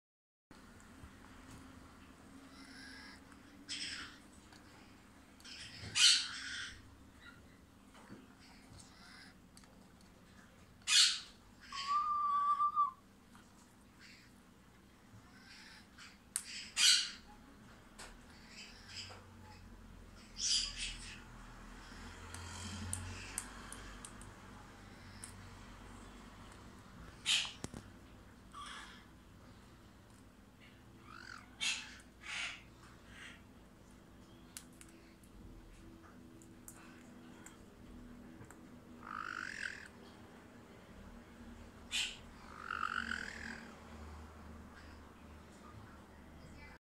Lía y Nano desgastando su pico
Caiques picando madera para ayudar a desgastar su pico